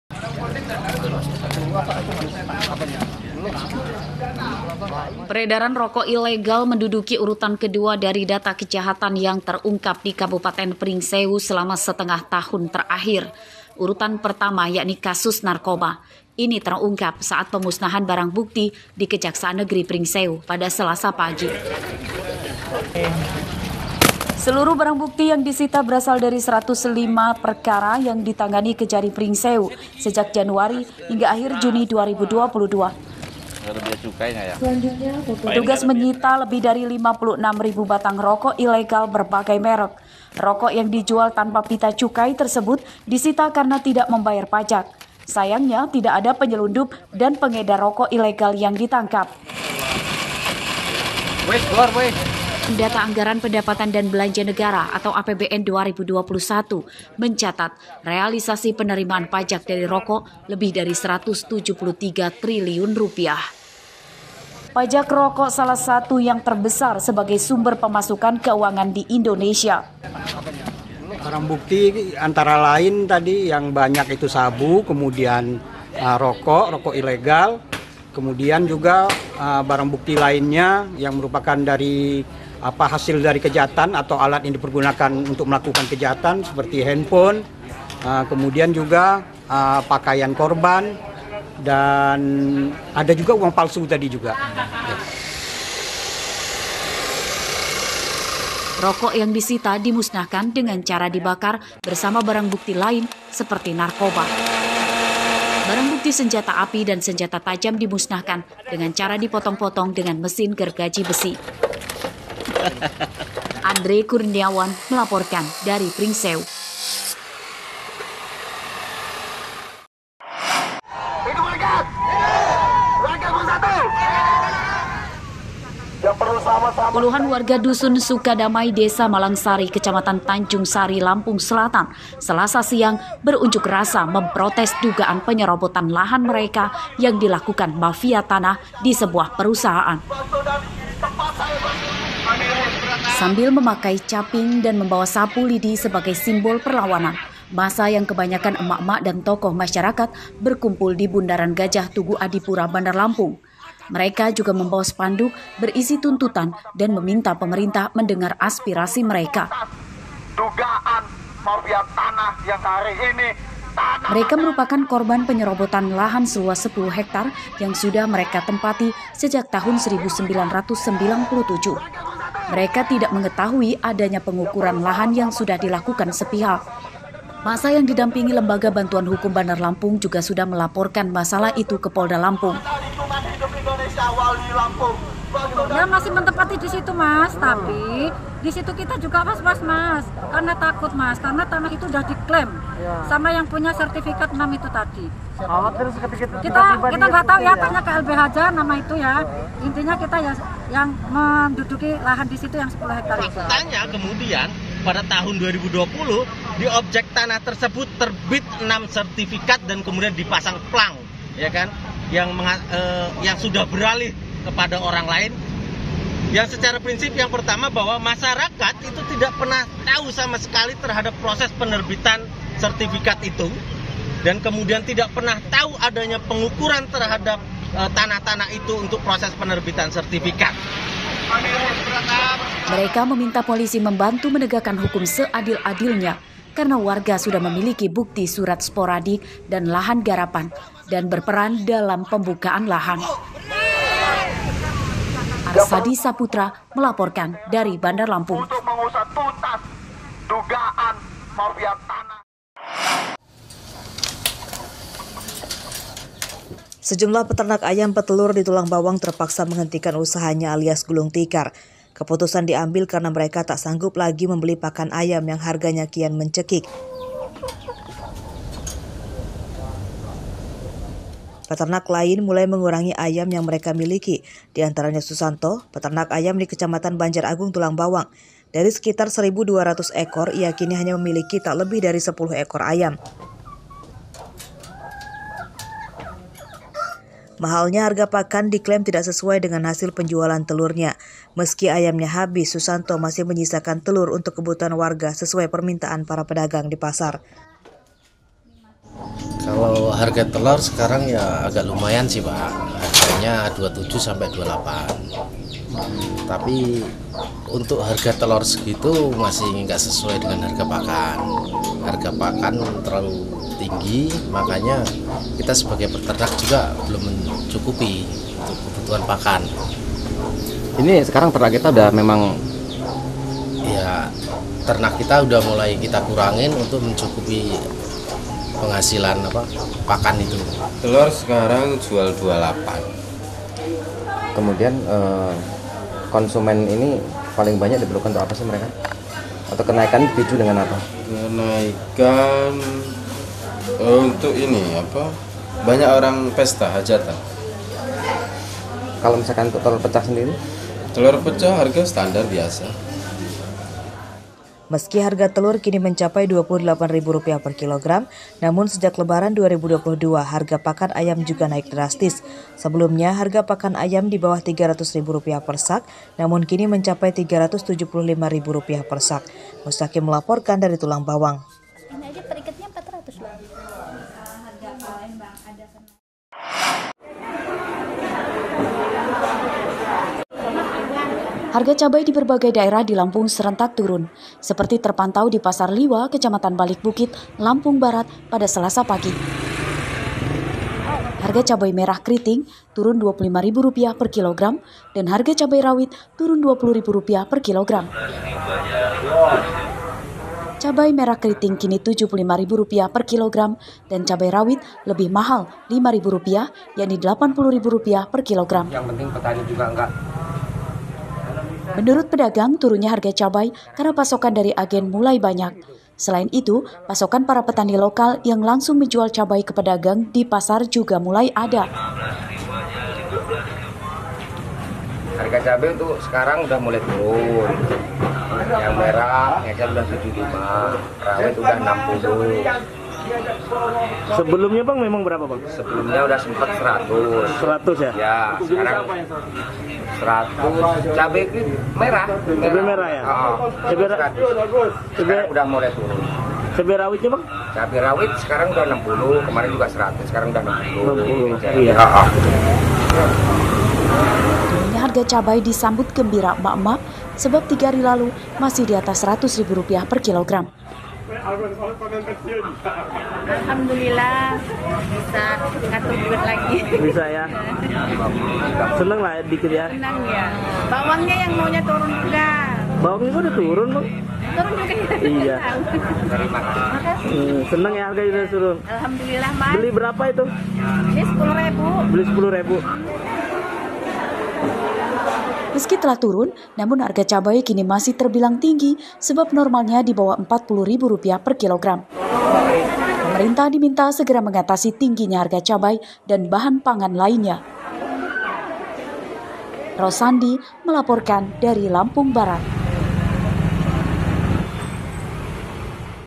Warga Demo Minta Pemerintah Selesaikan Masalah Sengketa Tanah
Berita Lampung, 20 Juli 2022 (2)